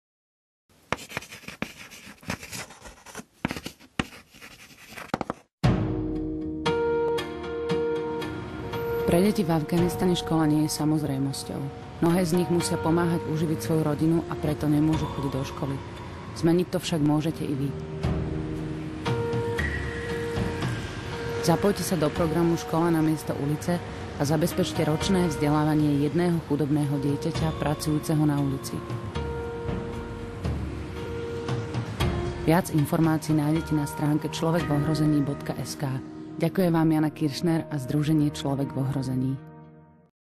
Adopčný program Škola namiesto ulice pre deti v Afganistane
Škola pre deti v Afganistane nie je samozrejmosťou. Tisícky z nich, často siroty alebo polosiroty, pracujú na ulici a predajom drobného tovaru sa snažia pomôcť uživiť svoju rodinu.
Hoci ako každé deti majú svoje sny a radi by sa stali lekármi, učiteľmi či vedcami, vymeniť školu za ulicu si nemôžu dovoliť - 20 dolárov, ktoré v priemere za mesiac na ulici zarobia, by ich rodinám bytostne chýbalo.
V rámci programu adopcií na diaľku, ktorý realizujeme od roku 2005 spolu s našim lokálnym partnerom - afgánskou organizáciou Aschiana - dostane rodina od darcu mesačný príspevok, aby dieťa nemuselo pracovať. V danom čase sa rodina zaviaže, že umožní dieťaťu vzdelávanie v štátnej škole.
Pracovníci občianskeho združenia Človek v ohrození sú s adoptovanými deťmi v pravidelnom kontakte. Dvakrát ročne darcovia dostanú fotografie a listy od adoptovaných detí a ich študijné výsledky.
Váš príspevok vo výške 200 € zabezpečí, aby afganské dieťa mohlo po celý rok namiesto práce na ulici navštevovať školu - a hlavne - dáte mu nádej na lepší život. (Číslo účtu: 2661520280/1100, VS: 1114, popis transakcie: Vaše meno.)